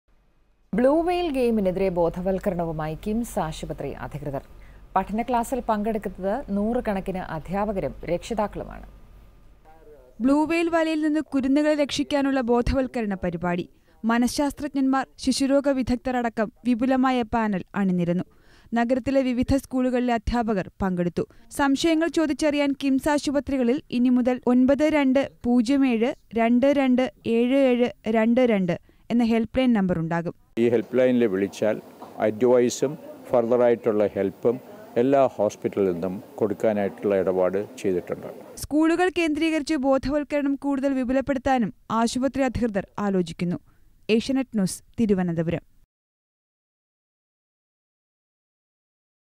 KIMS Hospital conduct against Blue Whale game
Know About Blue Whale Suicide Game awareness programme by KIMS hospital